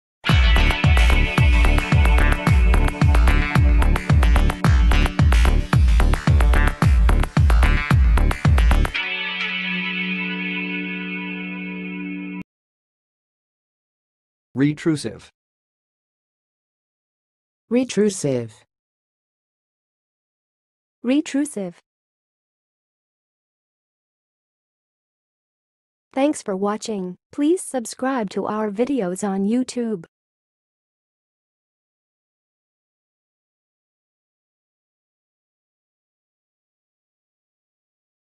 How to say "retrusive"! (High Quality Voices)
Watch in this video how to say and pronounce "retrusive"!